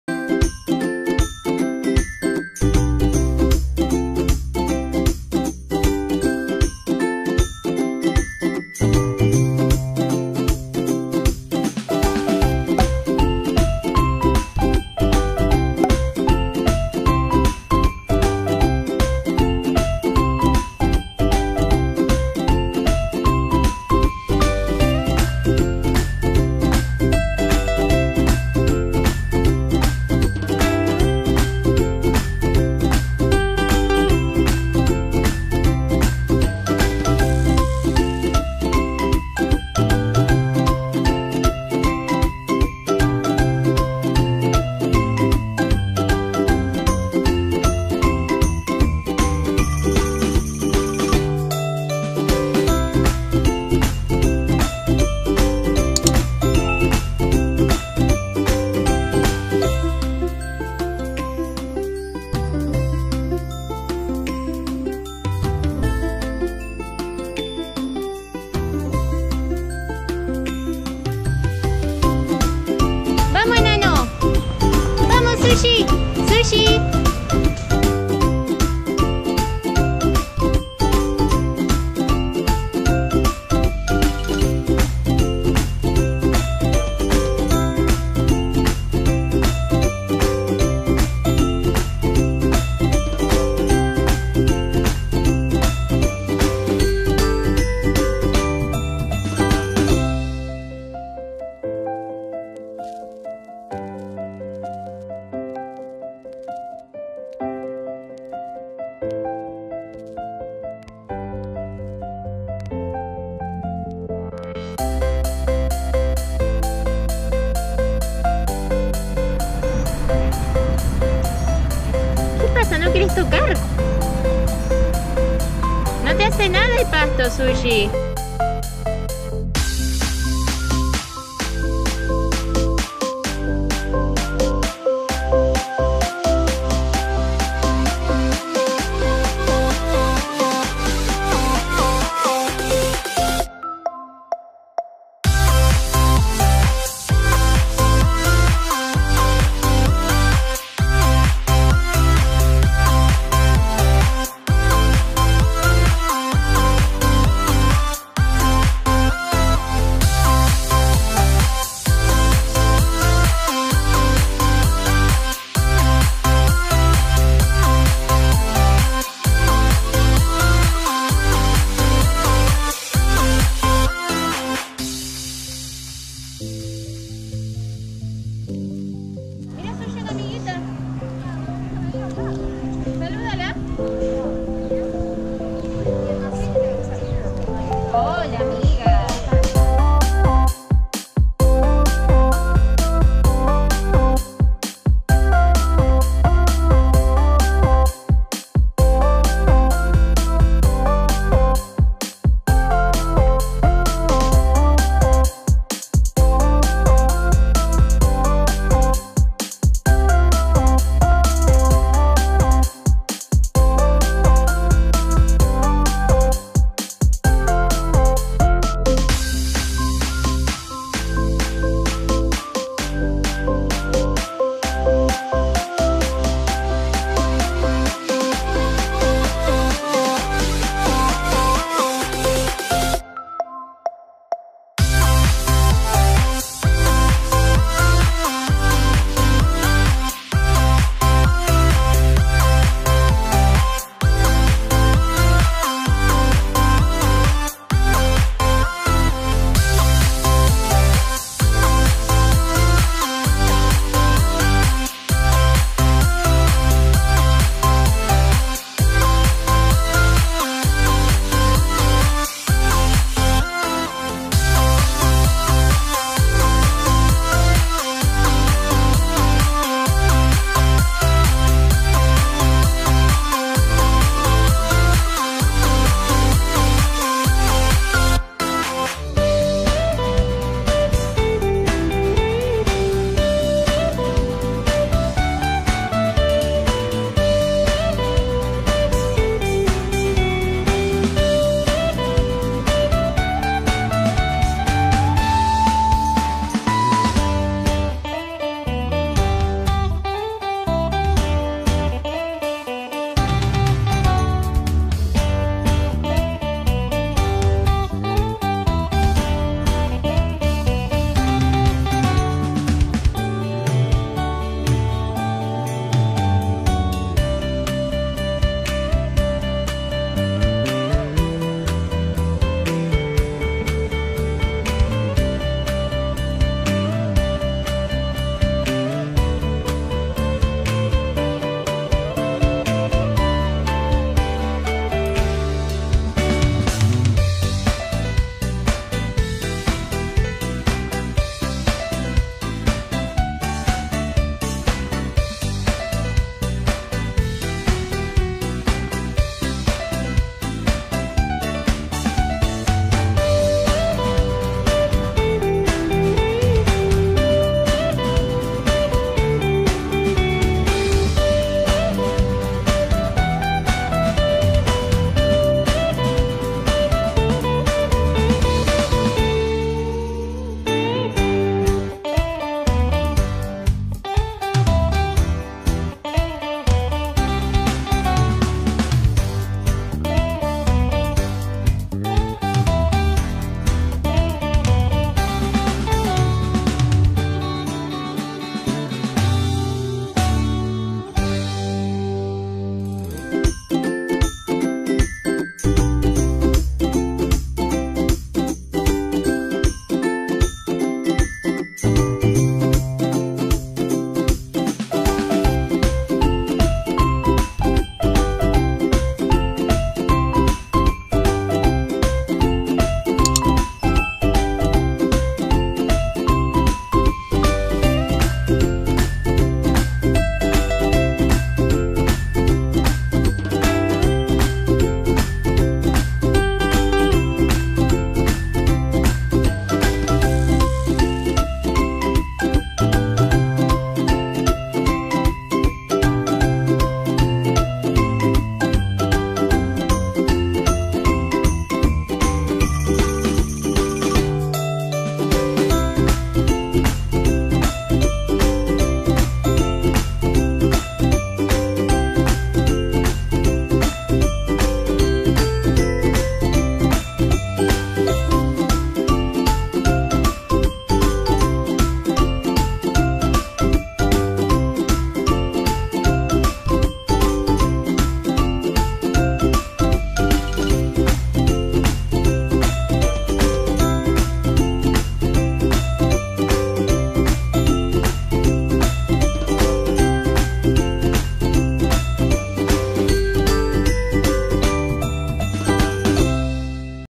SHIBA INU primer PASEO de mi CACHORRO | SHIBA INU PUPPY'S FIRST WALK - vlog WITH SUBTITLES
Sushi, mi cachorro Shiba inu reacciona a su primera caminata por el parque.
Sushi, my Shiba inu puppy reacts to his first walk in the park.
Sushi tiene cuatro meses y ya completó su calendario de vacunación para cachorros así que empezó a salir a pasear por el parque.
En este vlog con subtítulos te mostramos como fue si primer día y sus primeras reacciones. Todo era nuevo para él, al principio tuvo miedo pero luego se fue soltando.
Empezamos a salir todos los días y cada vez le gusta más.
Si tenes un SHIBA INU queremos que nos cuentes como fue su primer salida.
Qué otros videos te gustaría que hagamos??